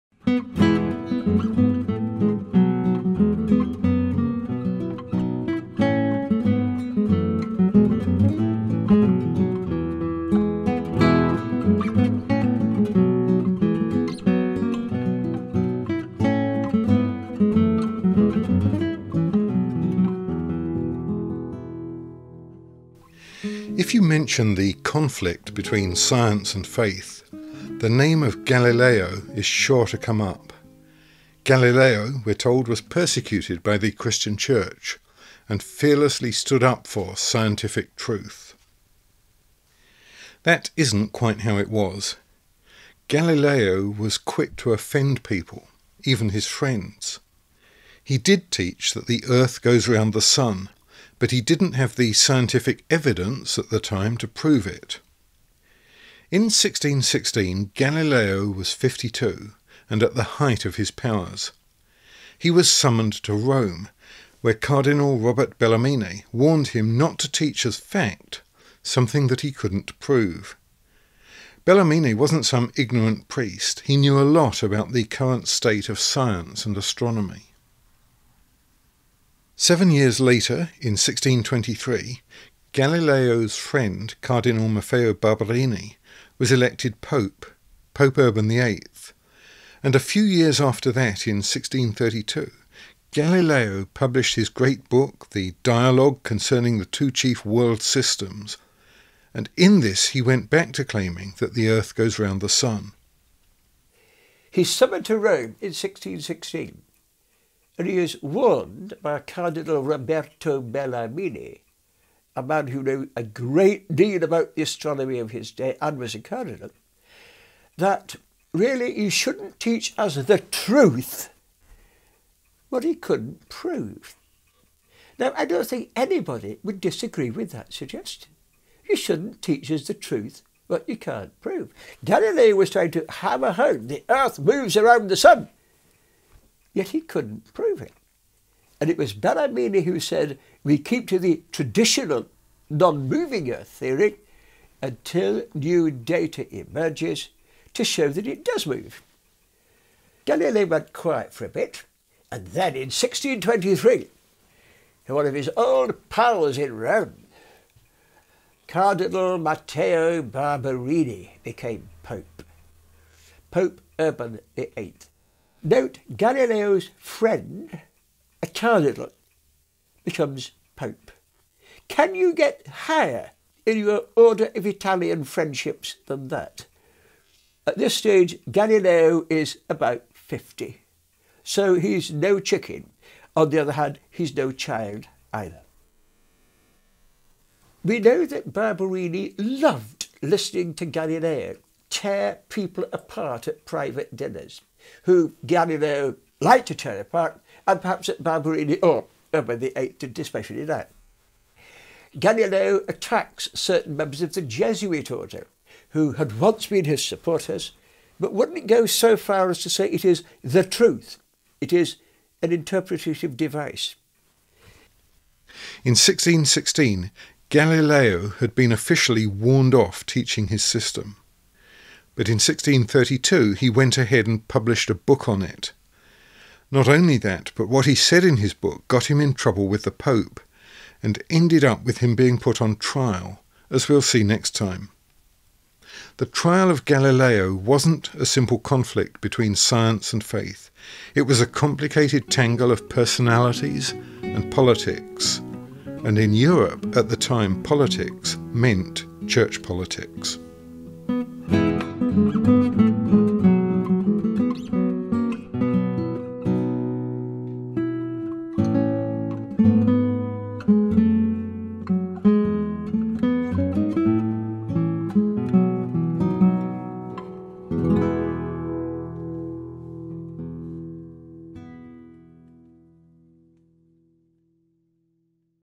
Why did Galileo get in trouble? (1616 - 1632) (Bad Faith #08)
In 1616, Galileo was summoned to Rome, where Cardinal Robert Bellarmine warned him not to teach as fact something that he couldn’t prove.  But in 1632 (with one of his friends now the Pope), Galileo published his book, the ‘Dialogue Concerning the Two Chief World Systems.‘ In it, he once again claimed that the Earth goes round the Sun – despite having been warned off claiming this until there was more evidence.